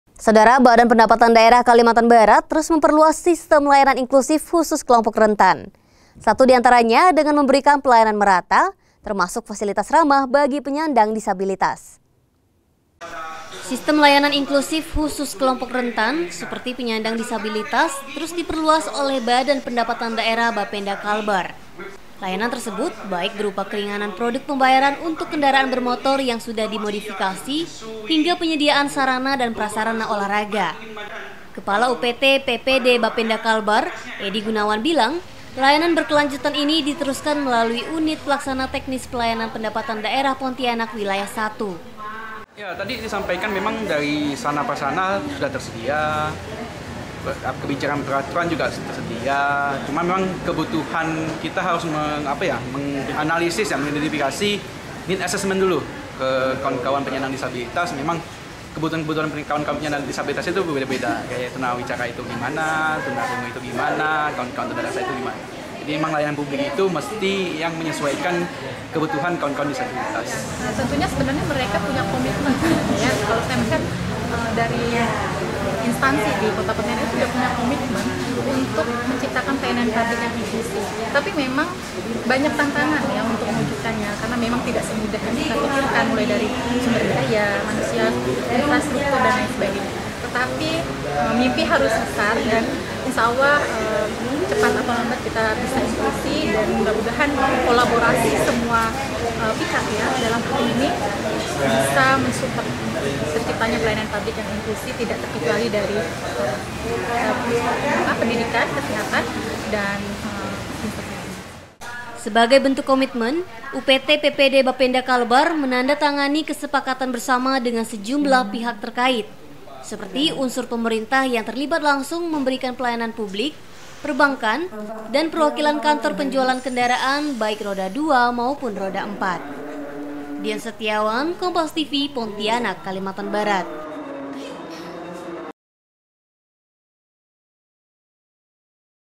Bapenda Kalbar Perluas Sistem Layanan Inklusif Kelompok Rentan dengan Pelayanan Merata
PONTIANAK, KOMPAS.TV - Bapenda Kalbar Perluas Sistem Layanan Inklusif Kelompok Rentan dengan Pelayanan Merata